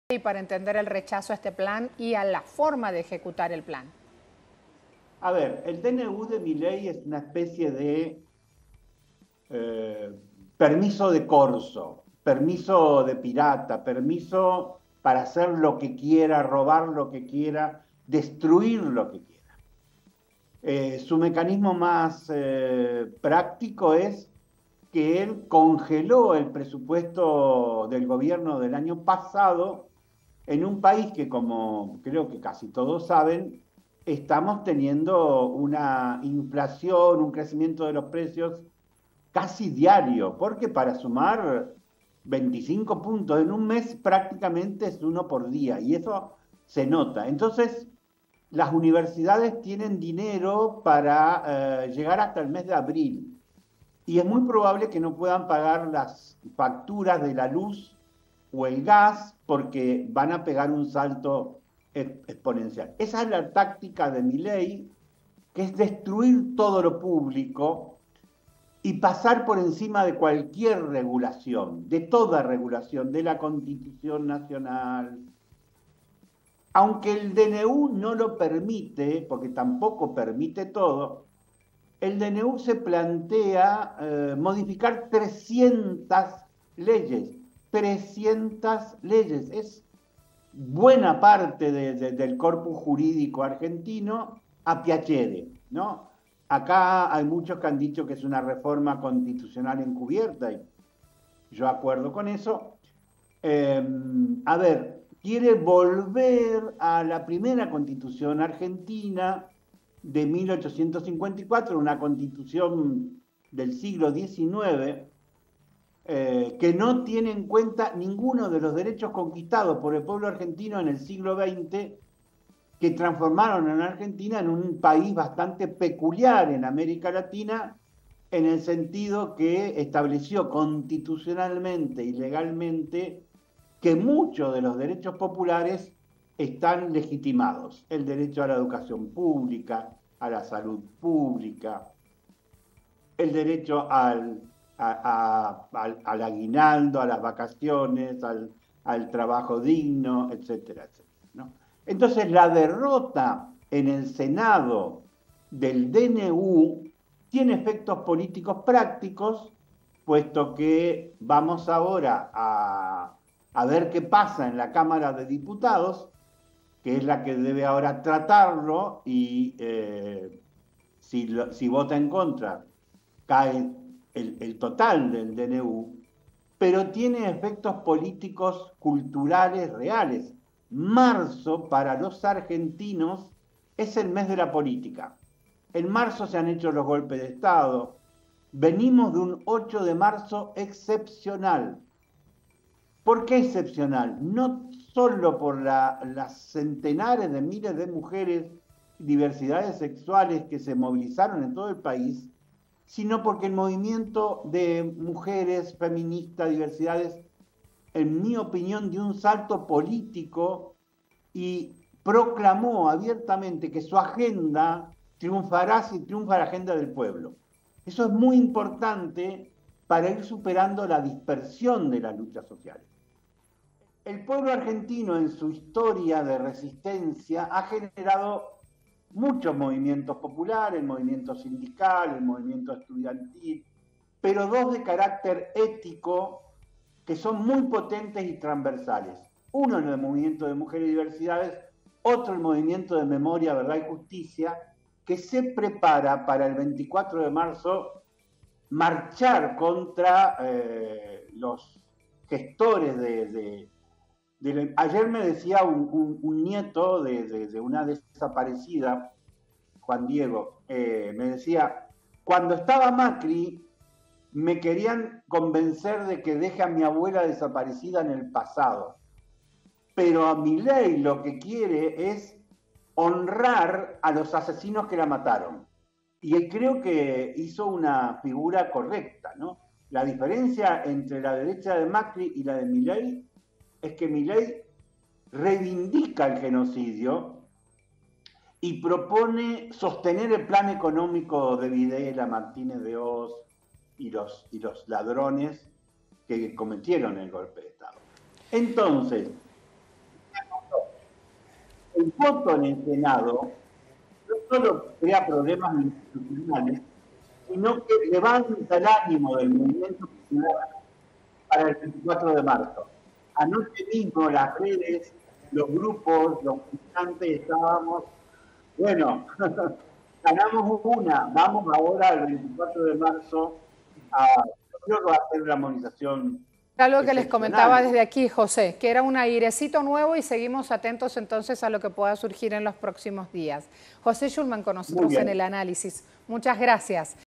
Jose Schulman en TeleSUR: "El DNU de Milei es un permiso de pirata"
En "Libres les queremos" compartimos la entrevista que le realizaron a José Schulman de la Coordinadora Americana por derechos de los pueblos.
José Schulman Representante de la Coordinadora Americana por los Derechos de los Pueblos comparte su opinión sobre el rechazo a la propuesta del Presidente Javier Milei. teleSUR